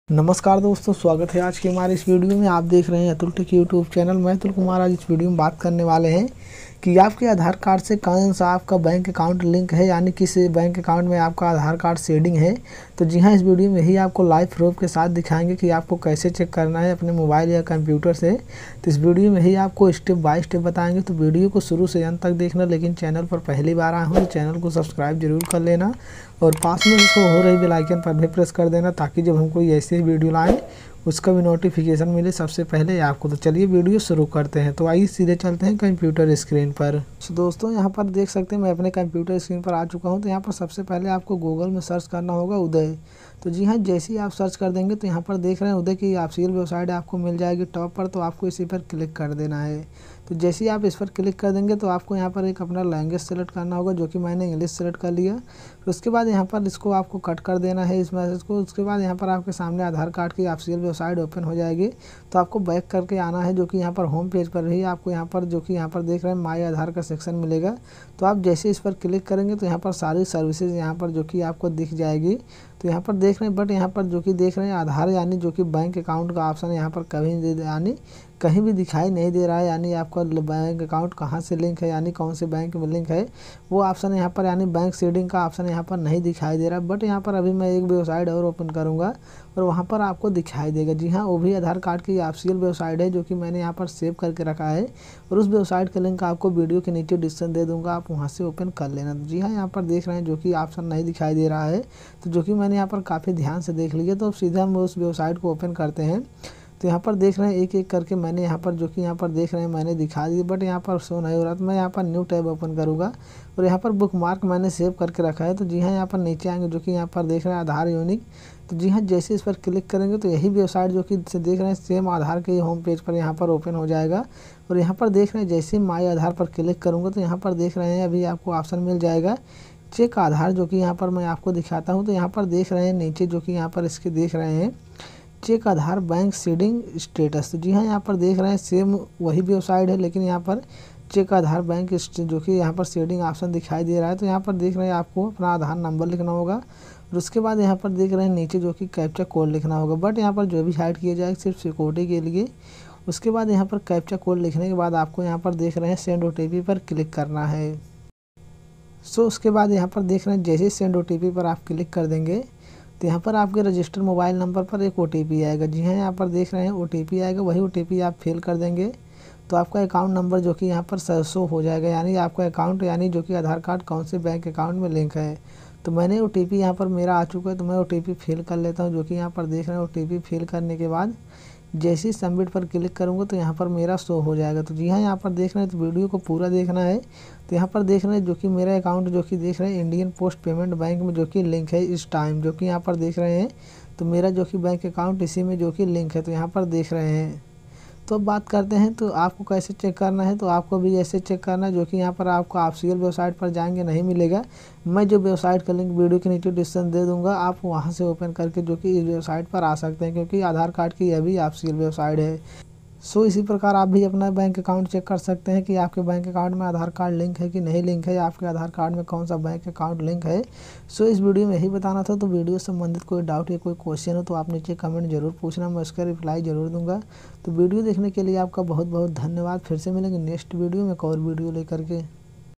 How to check Aadhaar Bank Seeding Status | आधार बैंक अकाउंट से कैसे सीड करायें | Atul Tech
Hi friends

नमस्कार दोस्तो स्वागत है आपका हमारे यूट्यूब चैनल में Atul Tech में
दोस्तों आज हम इस वीडियो में बताएंगे आधार कार्ड में कौन सा बैंक अकाउंट लिंक है आधार कार्ड में कौन सा बैंक Seeding है या आपके आधार कार्ड में बैंक अकाउंट Seed है कि नहीं प्लीज वीडियो में ही बताएंगे और साथ ही आपको लाइव चेक करके दिखाएंगे तो वीडियो शुरू से अंत तक देखना

और वीडियो से संबंधित कोई समस्या है तो आप कमेंट में पूछ सकते है

like shear comment

thanks for whaching